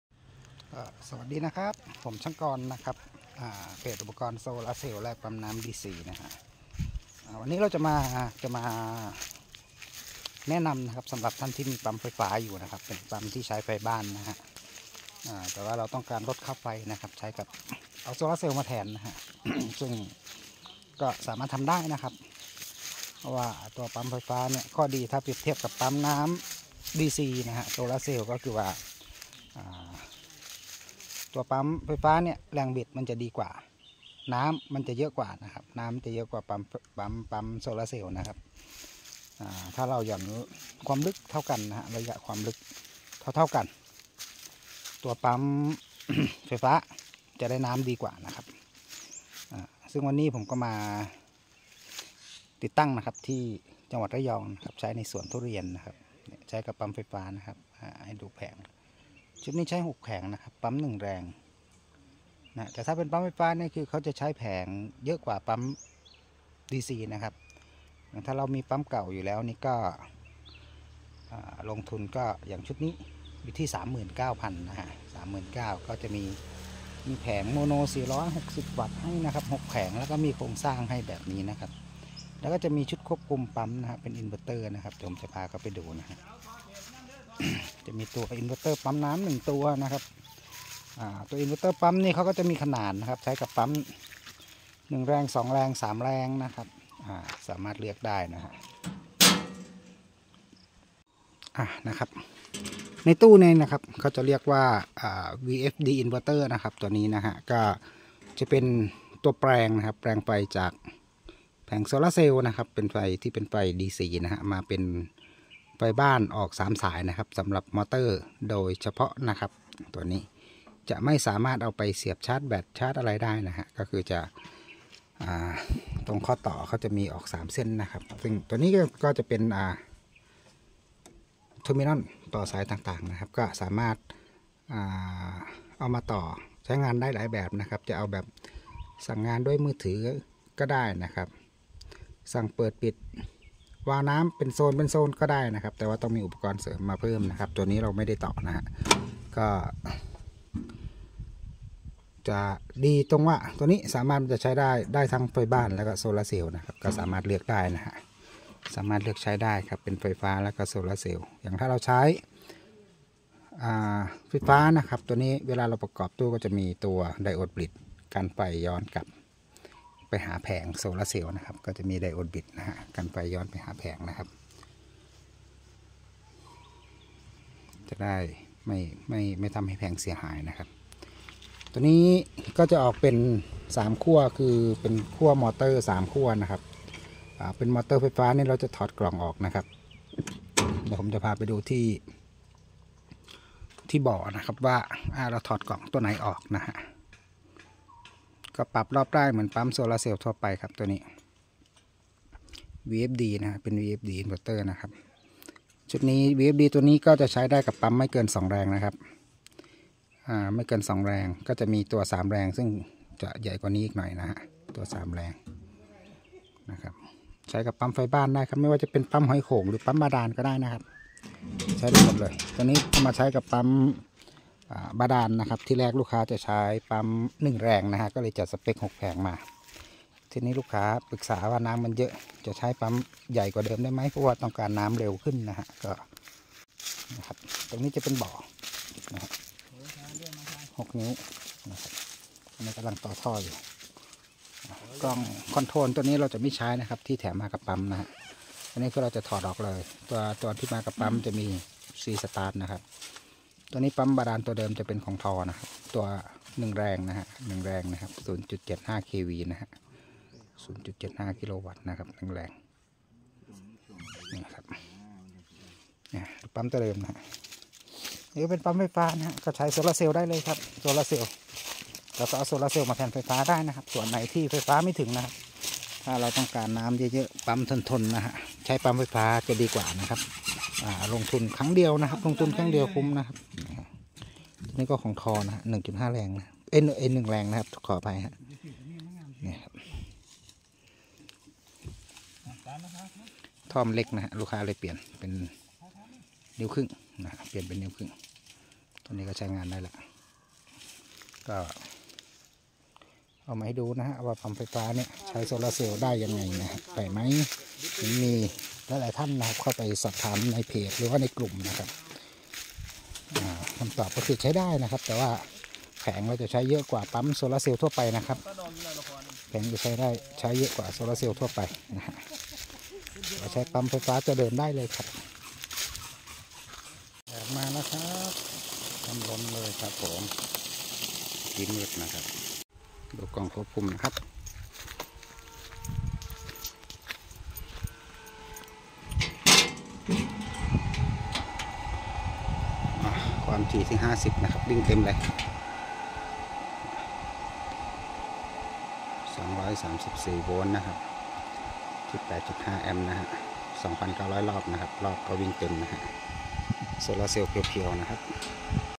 ปั้มไฟฟ้าใช้โซล่าเซลล์ได้
ใช้โซล่าเซลล์ แทนไฟฟ้า สูบน้ำทำสวนทุเรียน กับปั้มตัวเดิมที่ใช้ไฟฟ้าอยู่